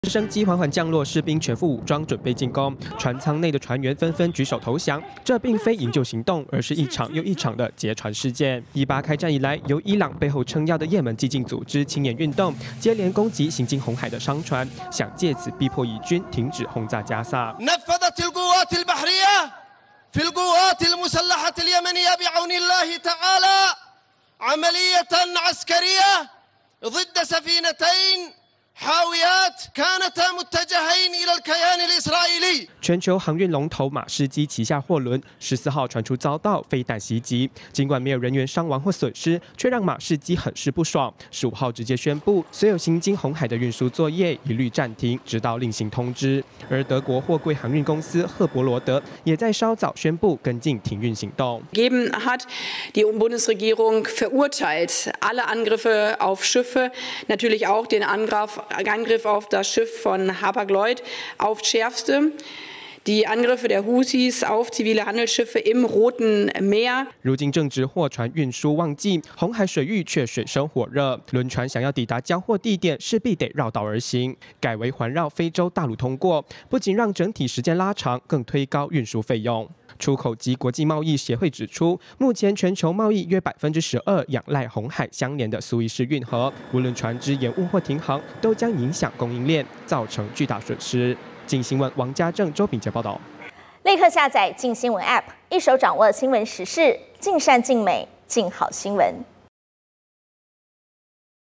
紅海動盪！馬士基停航紅海 德國赫伯羅德也跟進｜鏡新聞
有伊朗在背後撐腰的，葉門激進組織青年運動，最近頻繁攻擊通往紅海的貨船，想要藉此逼迫以巴停火。全球航運巨擘馬士基昨日（12/15），在自家貨船被襲擊後宣布，將暫停一切紅海的航運作業，而德國貨櫃航運公司赫伯羅德稍後也跟進，宣布停航，由於此時正值航運旺季，恐怕會對全球供應鏈，造成負面影響。